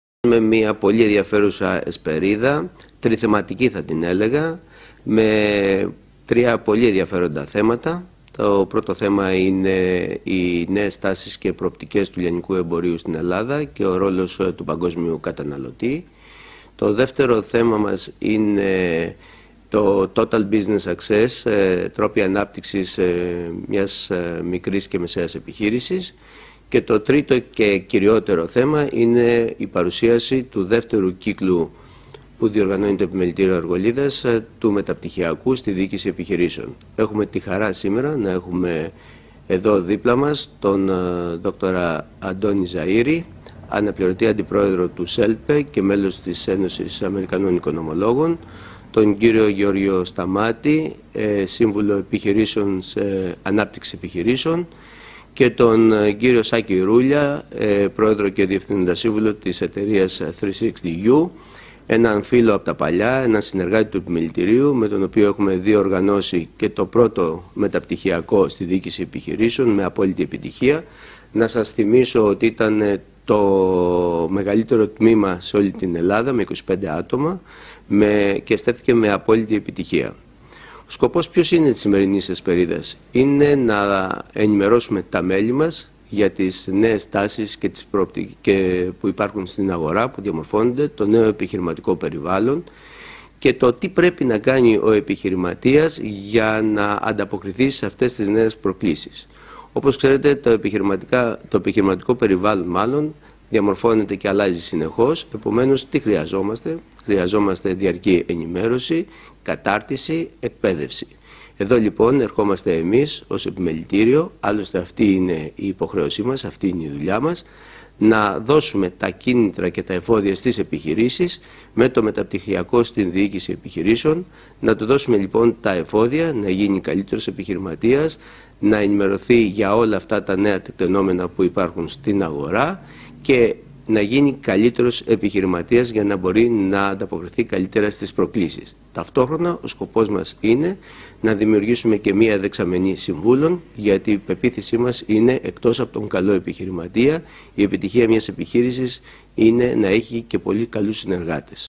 ArgolidaPortal.gr Επιμελητήριο Αργολίδας - Φ.Δαμούλος: Προοπτικές του Λιανικού Εμπορίου
(Βίντεο © eurokinissi-Βασίλης Παπαδόπουλος) Ο Πρόεδρος του Επιμελητηρίου Αργολίδας Φώτης Δαμούλος.  Ενημερωτική Εσπερίδα ,του Επιμελητηρίου Αργολίδας, με θέμα «Οι νέες τάσεις & προοπτικές του Λιανικού Εμπορίου στην Ελλάδα & ο Ρόλος του Παγκόσμιου Καταναλωτή», την Τετάρτη, 26 Φεβρουαρίου 2020 στην αίθουσα εκδηλώσεων του Επιμελητηρίου